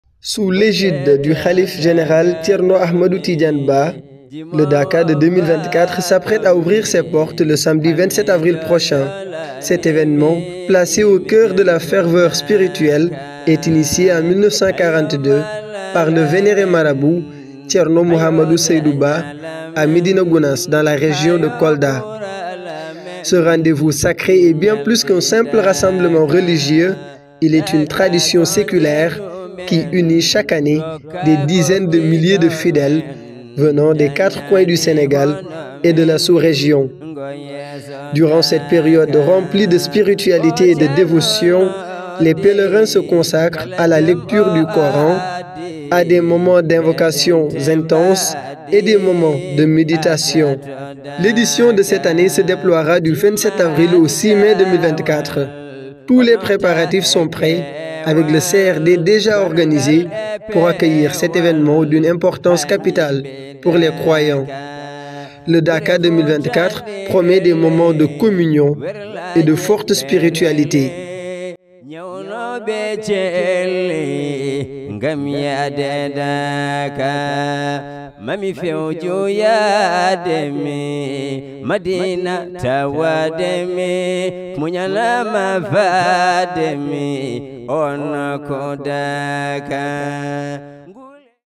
AMANI TV vous fera suivre l'édition 2024 du DAAKAA de Médina Gunaas
Activez les notifications Merci d'avance